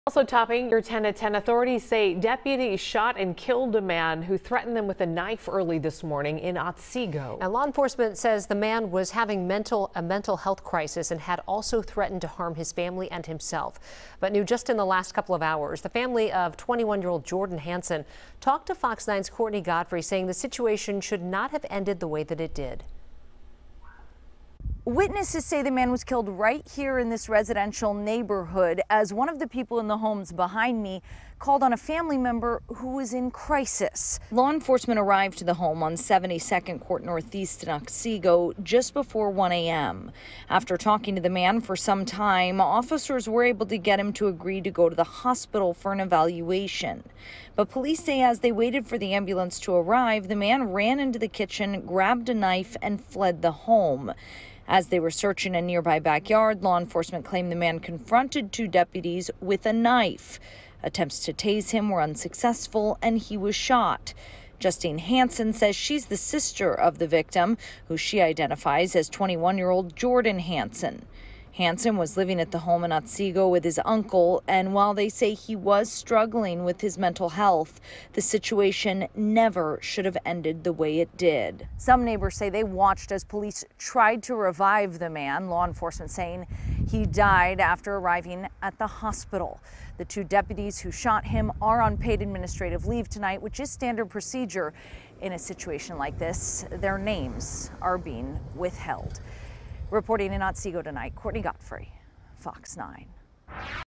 21-year-old man dies after being shot by Wright Co. deputies | FOX 9 KMSP
Deputies with the Wright County Sheriff’s Office fatally shot a man who they say was "having mental health challenges" and allegedly threatened them with a knife early Sunday morning.